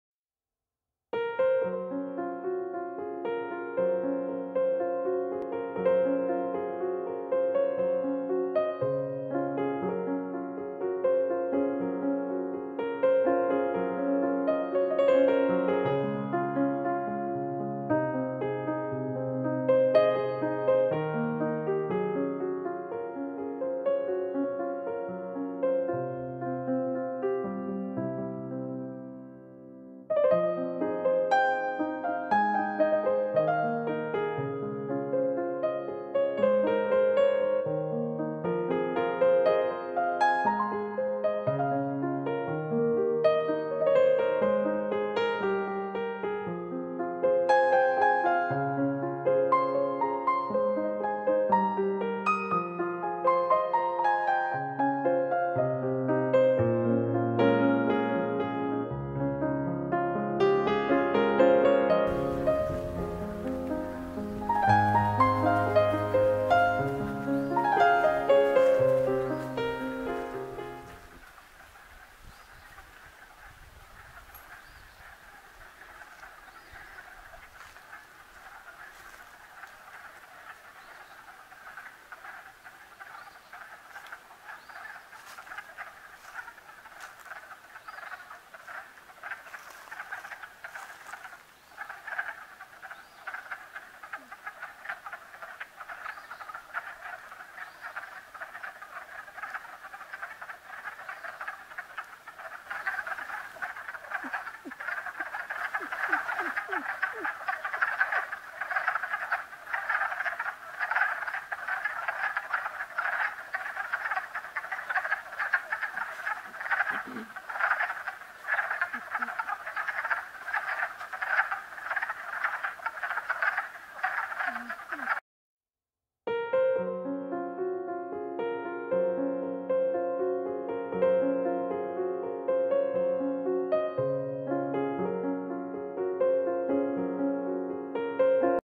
MILLSTONE TRAILS
Old and abandoned granite quarries are now used as walking and mountain biking trails.